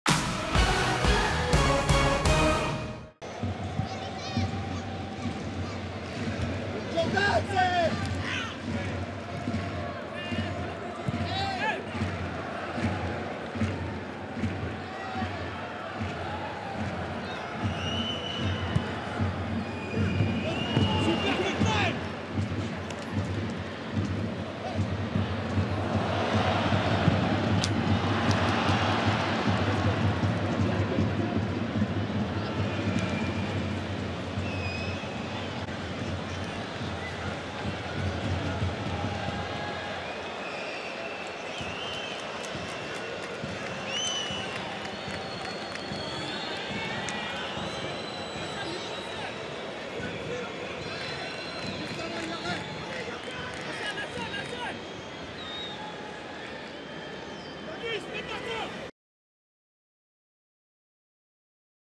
But Franck HONORAT (90' +2 - SB29) OGC NICE - STADE BRESTOIS 29 (2-1) 21/22
"OGC NICE - STADE BRESTOIS 29 (2-1) But de Franck HONORAT (90' +2 - SB29)  + Tous les buts de OGC NICE - STADE BRESTOIS 29 en vidéo.

Ligue 1 Uber Eats - Saison 2021/2022 - 9ème journée ALLIANZ RIVIERA - samedi 2 octobre 2021

Buteurs : Jean-Clair TODIBO (45' - OGCN) / Melvin BARD (61' - OGCN) / Franck HONORAT (90' +2 - SB29)